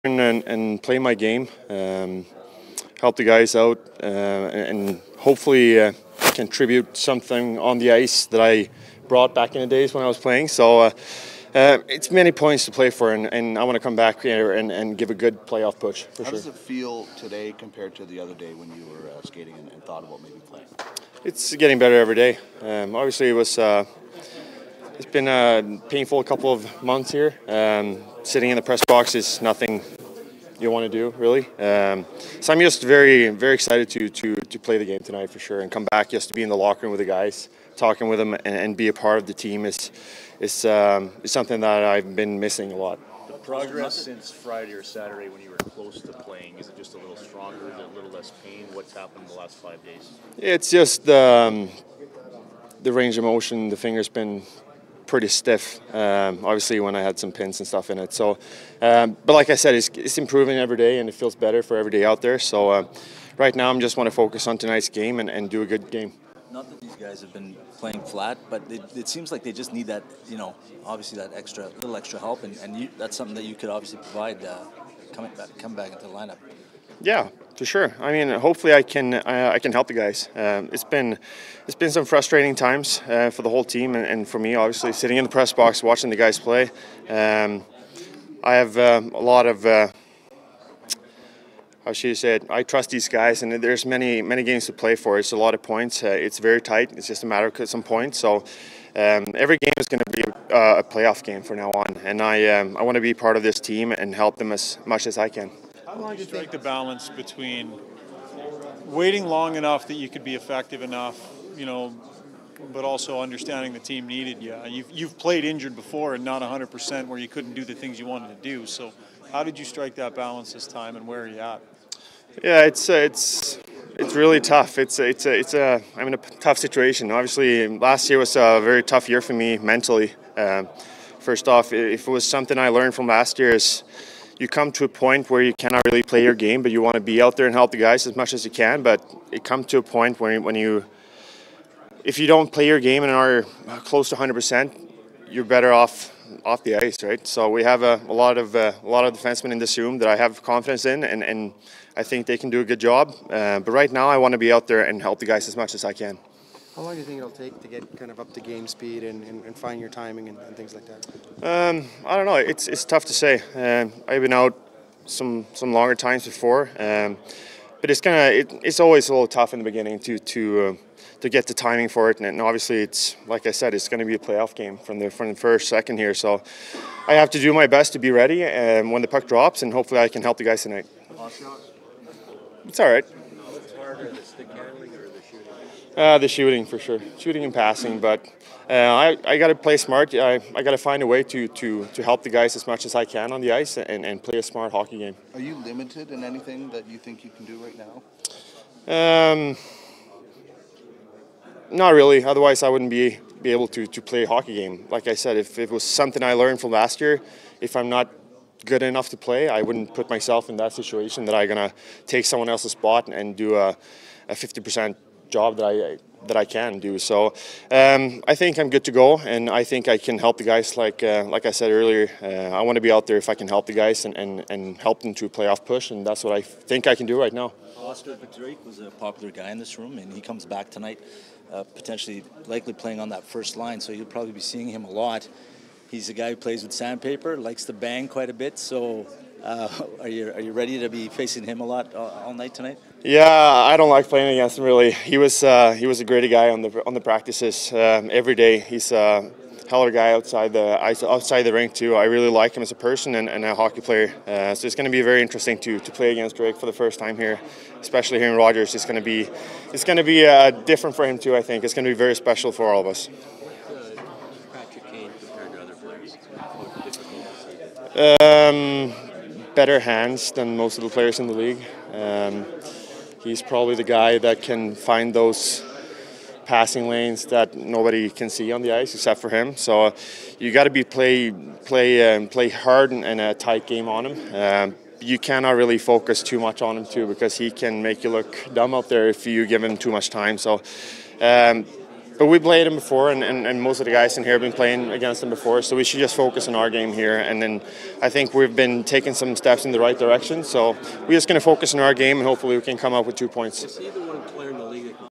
PRE-GAME RAW | Oscar Klefbom 02.05.19
Oscar Klefbom talks about returning to the lineup to play his first game since his hand injury.